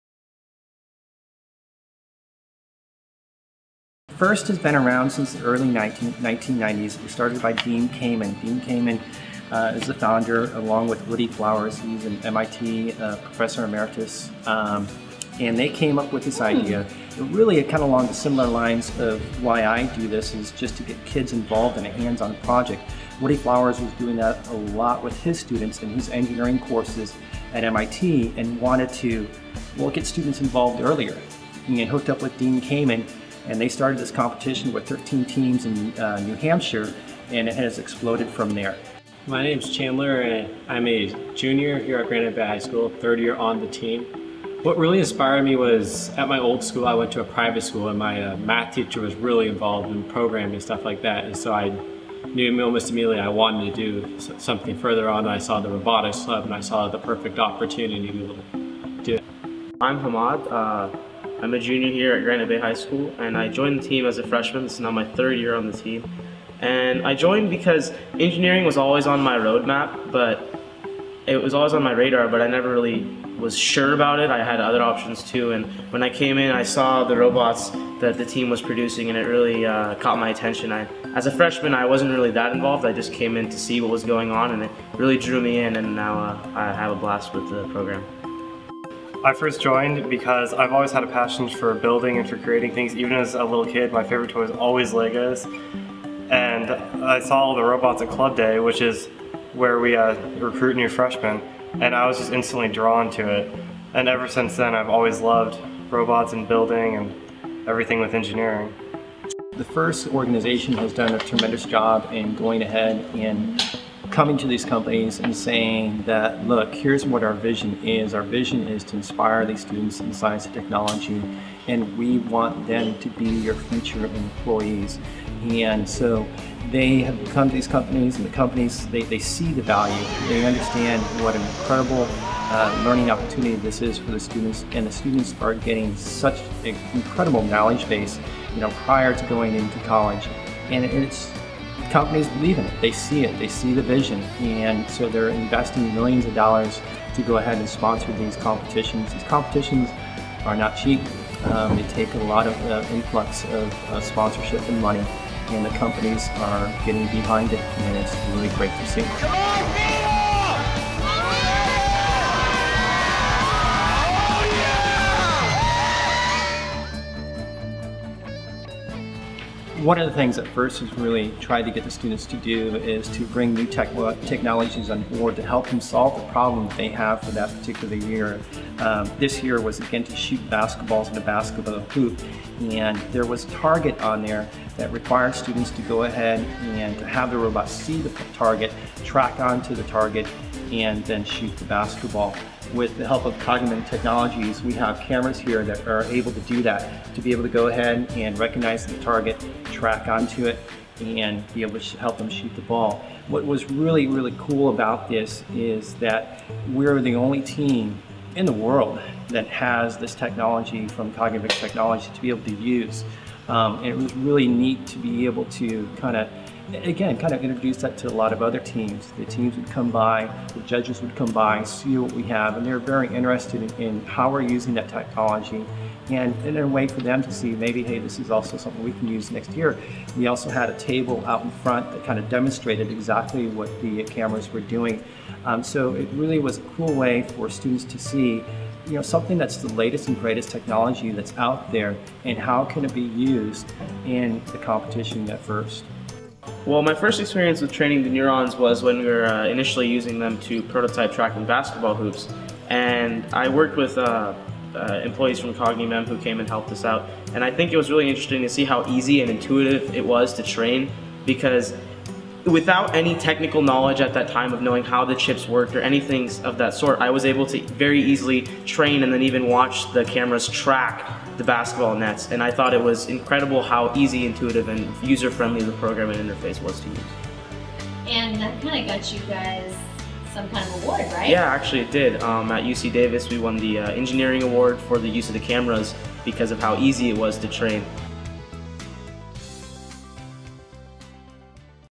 Robotics with Cognimem
A group of high school students apply Cognimem's CM1K neural network chip, at the FIRST robotics competition. The group received the award of Engineering Excellence for it's use of the technology.